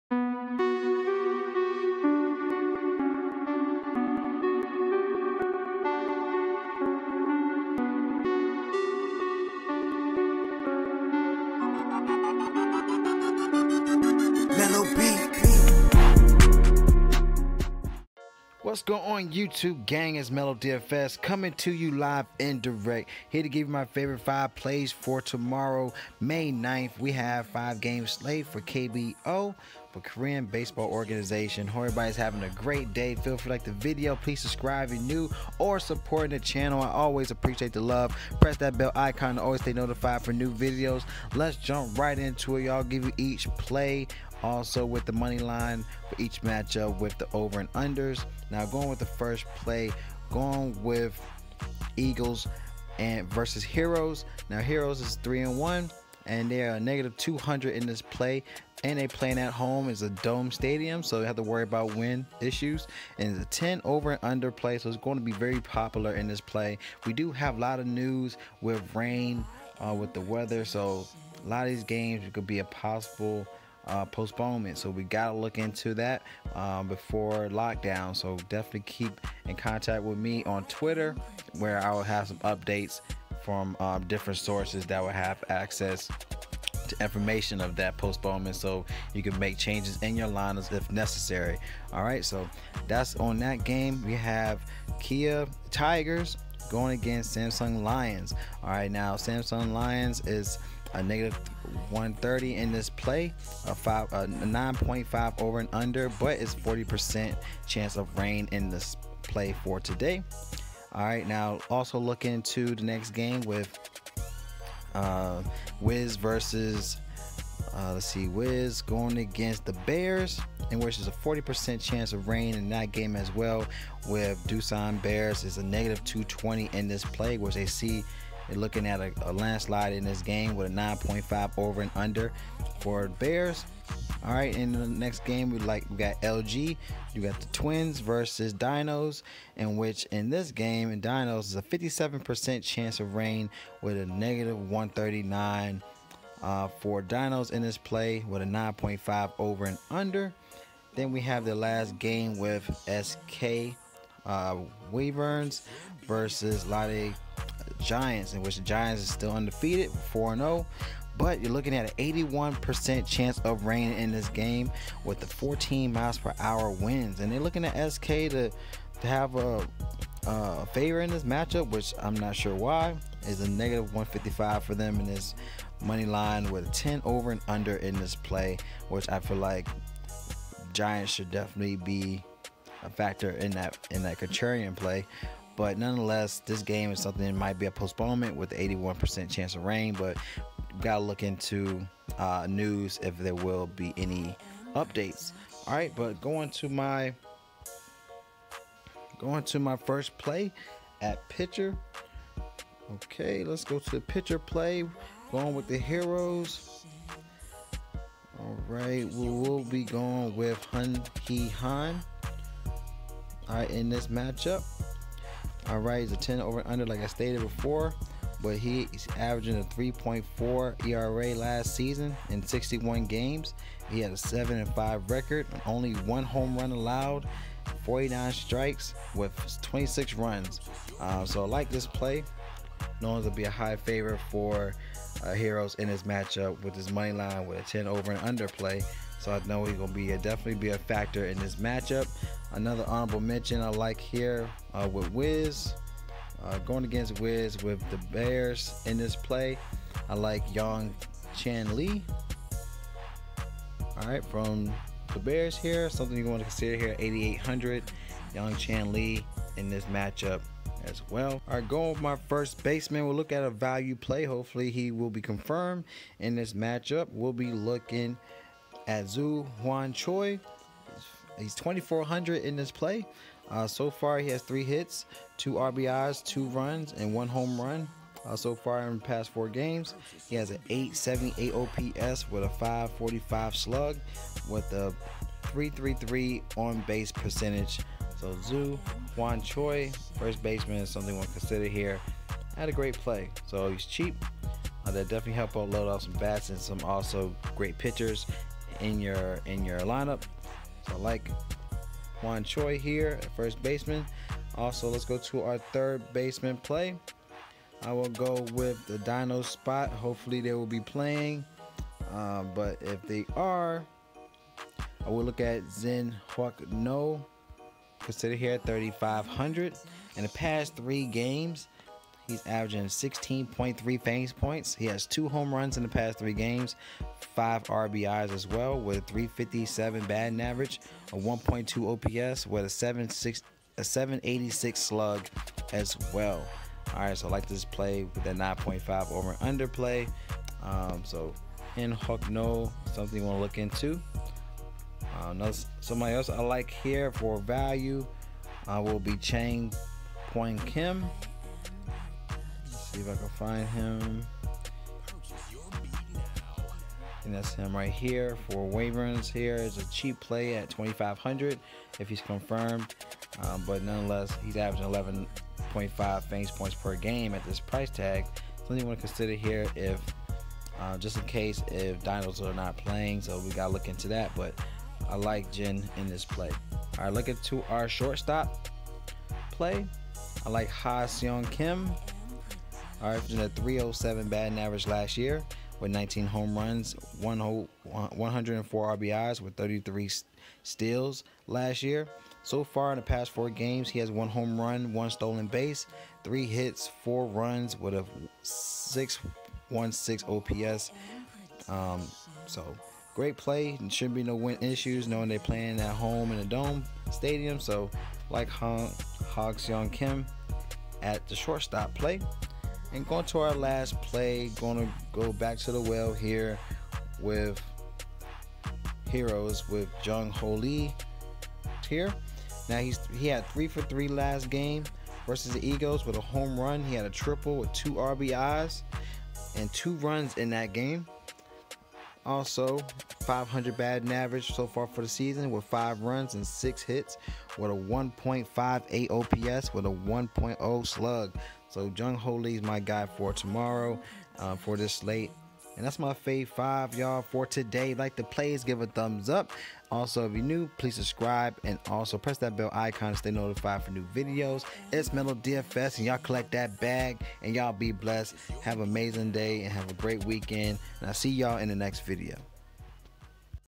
KBO DraftKings DFS Picks, FanDuel Picks 5/9/2020
M3lo's Favorite 5 picks for KBO MAIN SLATE!

Have taken the time to put in much research and trial lineups in place to further my daily fantasy criteria and looking forward to an exciting journey!

Will provide confirmed lineups/updated "Fav 5" before lockdown on Twitter, so ADD me @m3lorama